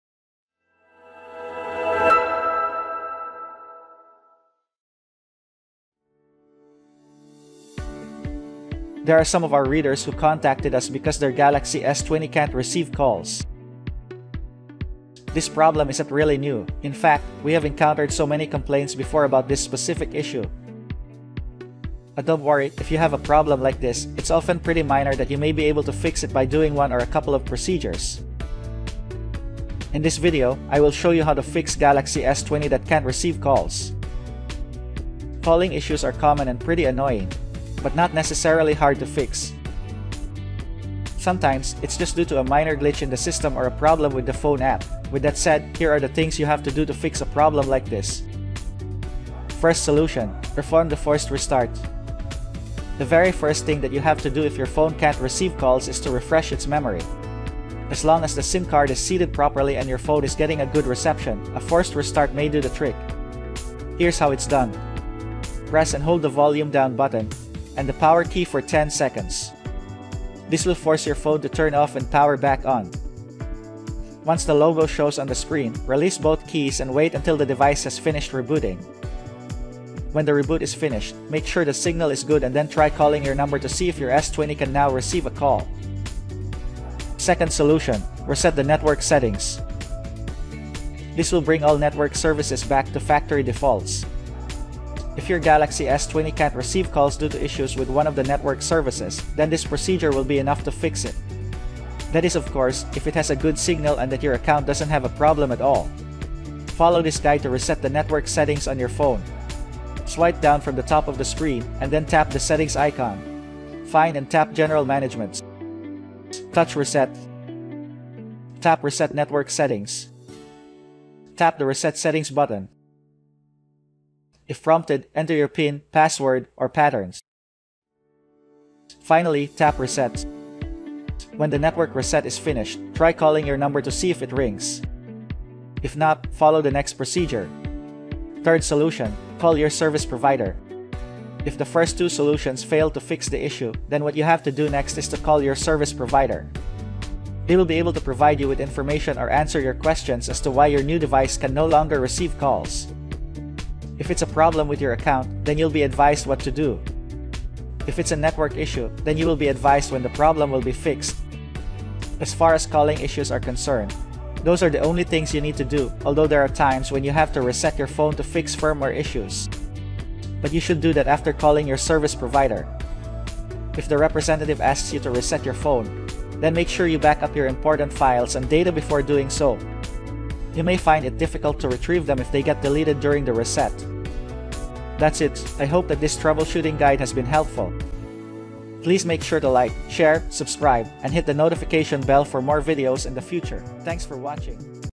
Galaxy S20 Can’t Receive Calls? Here’s The Fix!
In this video, I will show you how to fix Galaxy S 20 that can't receive calls.

00:49 Perform Forced Restart

01:27 Reset the network Settings

02:18 Call your service provider